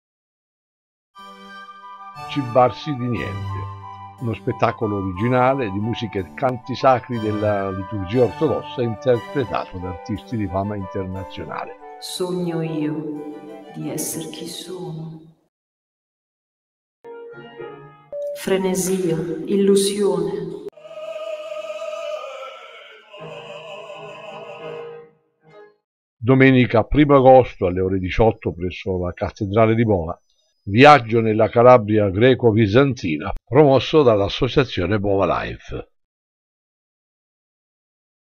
"Cibarsi di Niente", uno spettacolo originale di musiche e canti sacri promosso da "Bova Life"
Domenica 1° agosto alle 18:00 presso la Cattedrale di Bova - Viaggio nella Calabria "greco bizantina" promosso dall'Associazione Bova Life.
"Cibarsi di Niente", uno spettacolo originale di musiche e canti sacri della liturgia ortodossa, interpretato da artisti di fama internazionale, su testi originali e tratti dagli scritti di Luca, vescovo di Bova e dal Compendio della gloriosa e santissima vita e morte di Santo Leone Cittadino e Patrono della città di Bova, musiche originali e canti sacri della liturgia ortodossa.
👉 Interpreti:
▶ Francesca Tuscano: voce recitante
▶ Annarita Poliseno: recitazione
▶ Alexander Voronov: canto
▶ Fausto Tuscano: flauto